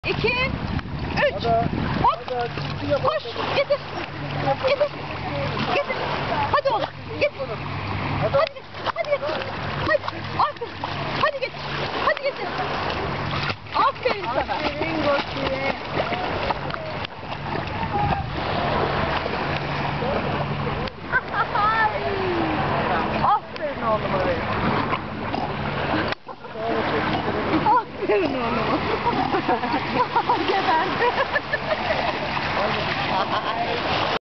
Gofi Denizde
22 Mayıs 2011 Heybeliada Pikniği
Gofi'nin deniz sefası